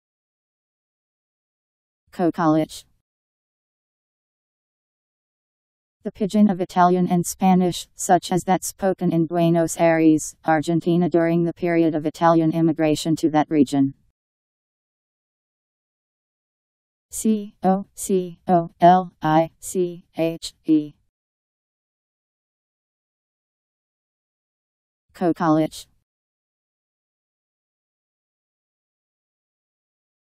Cocoliche Meaning
Video shows what Cocoliche means. The pidgin of Italian and Spanish, such as that spoken in Buenos Aires, Argentina during the period of Italian immigration to that region..  Cocoliche pronunciation. How to pronounce, definition by Wiktionary dictionary. Cocoliche meaning. Powered by MaryTTS